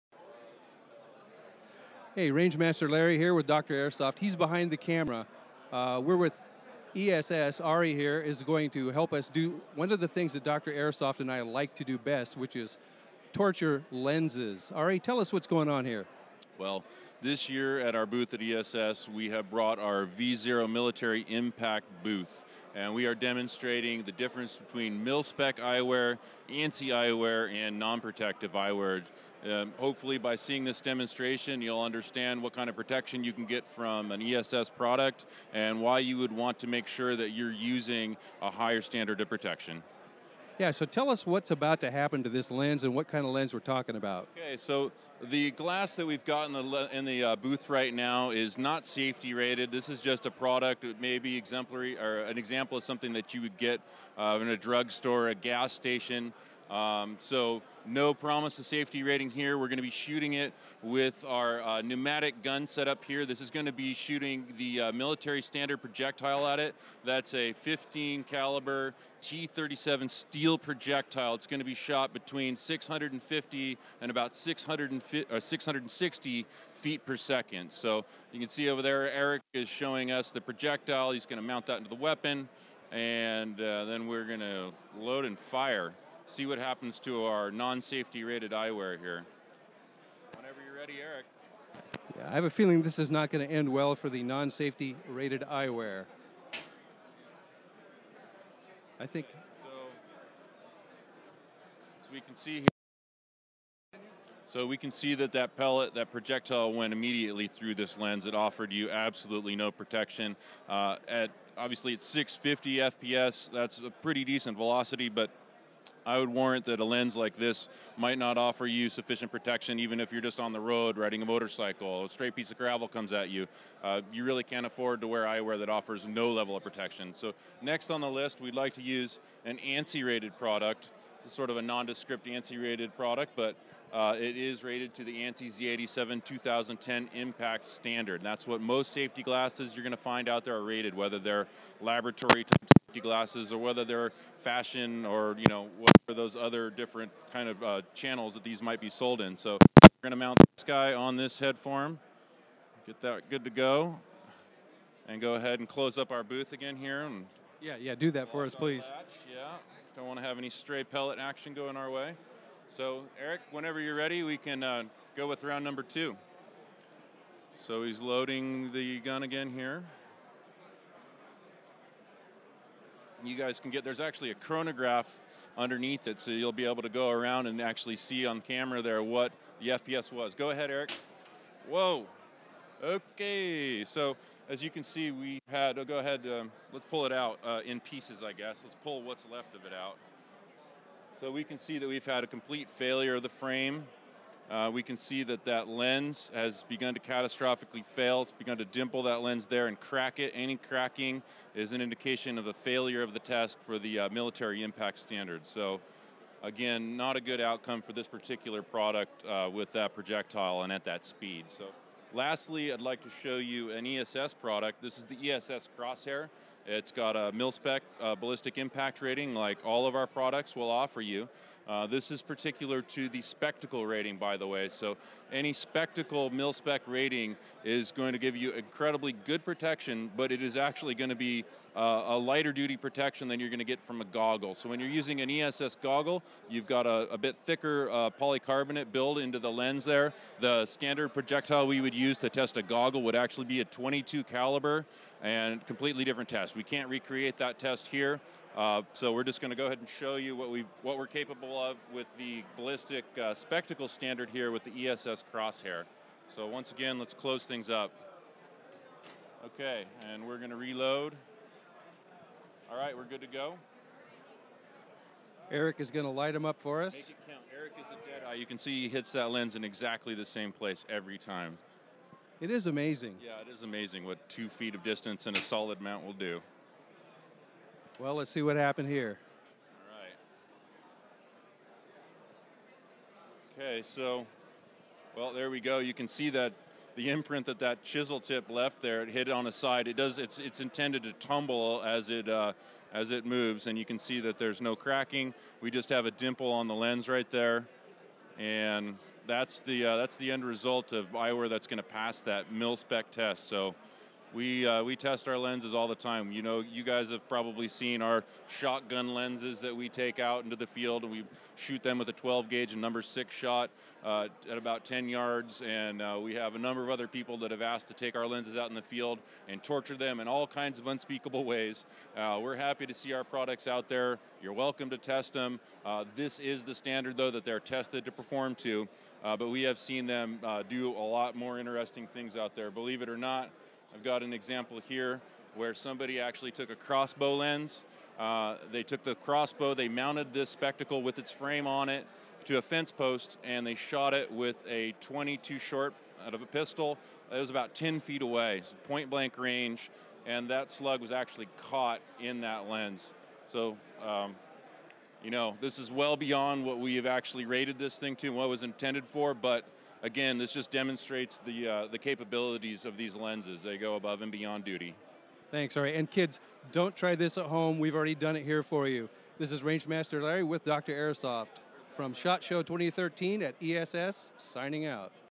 ESS Shoots Lenses at Shot Show 2013
ESS shows Airsoft Medicine how they test their lenses and competitors' lenses by shooting them at Shot Show 2013.  I created this video with the YouTube Video Editor